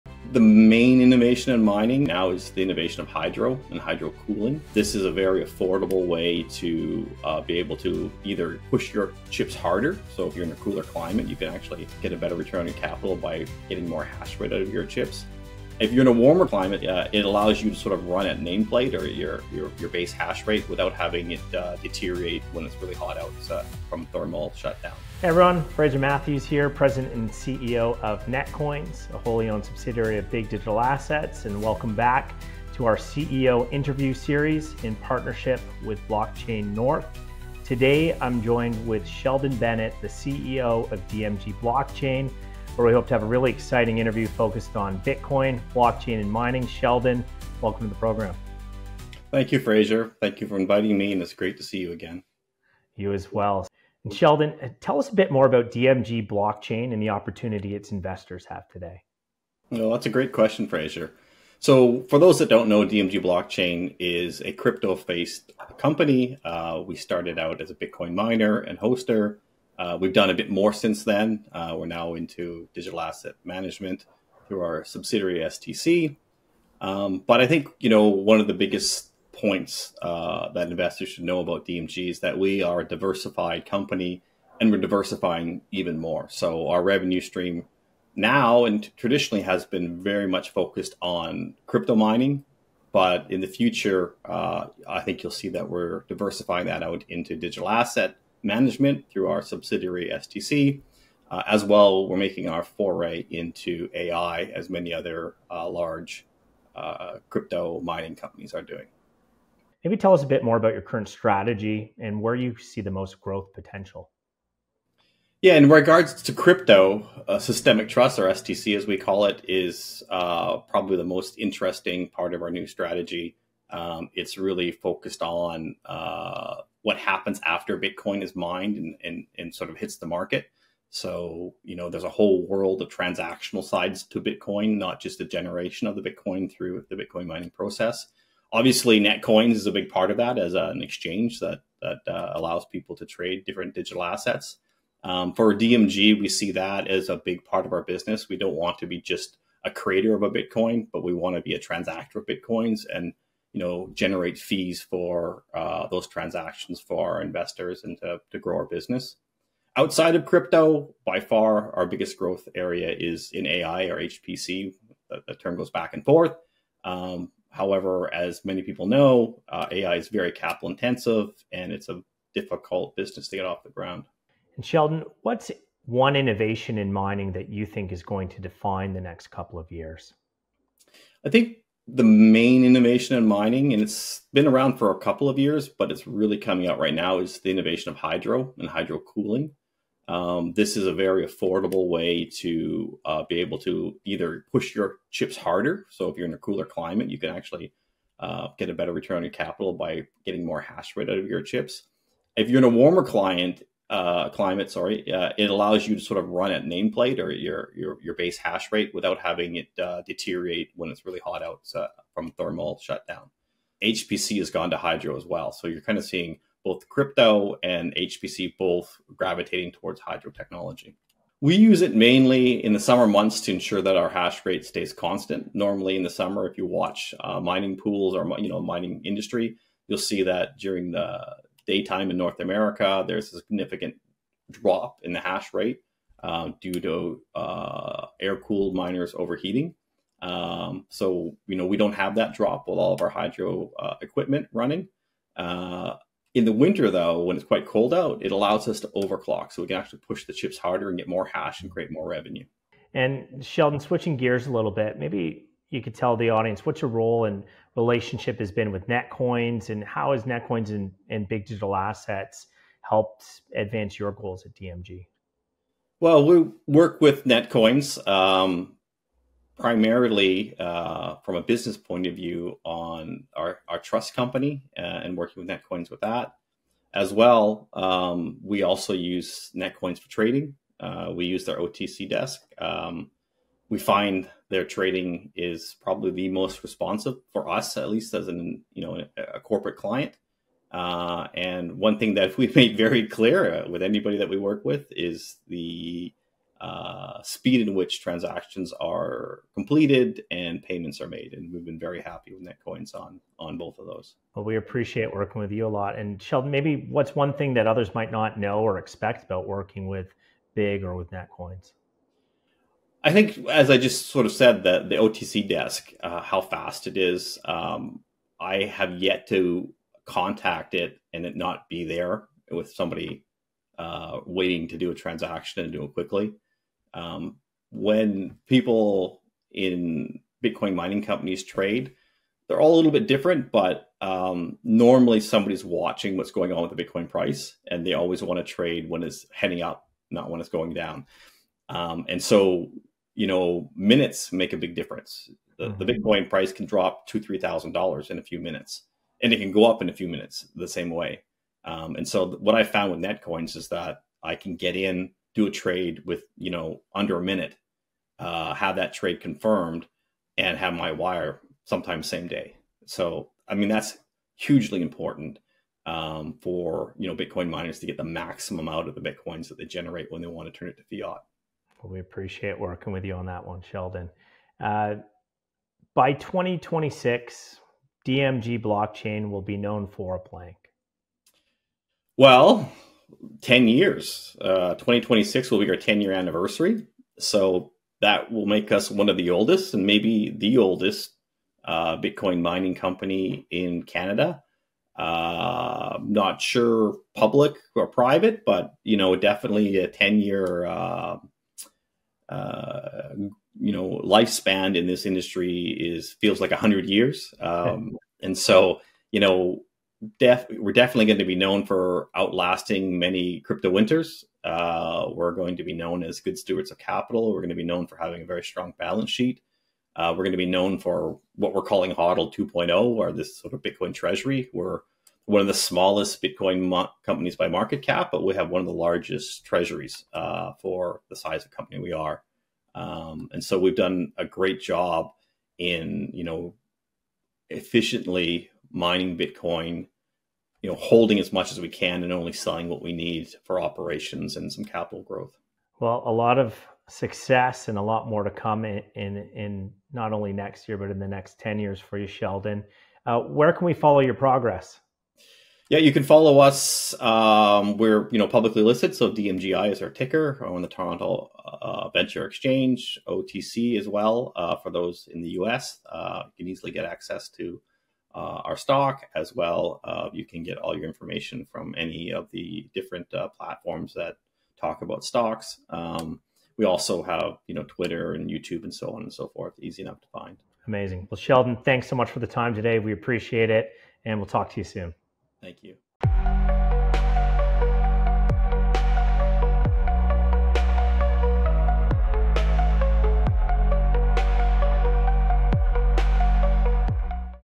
Hydro Cooling & Bitcoin Mining’s Next Evolution | Sheldon Bennett
What innovations are shaping the future of Bitcoin mining and blockchain infrastructure? ⚡

Sheldon Bennett, CEO of DMG Blockchain Solutions, joins Fraser Matthews, President & CEO of Netcoins, to discuss DMG’s strategy in mining, digital asset management, and AI. From pioneering hydro cooling technology to building one of Canada’s strongest Bitcoin treasuries, Sheldon shares how DMG is navigating crypto winters, diversifying revenue streams, and scaling into AI and HPC infrastructure.

This conversation is part of our partnership with Netcoins & BIGG Digital Assets.

📢 Thank you for your foundational support and commitment to Canada’s blockchain future.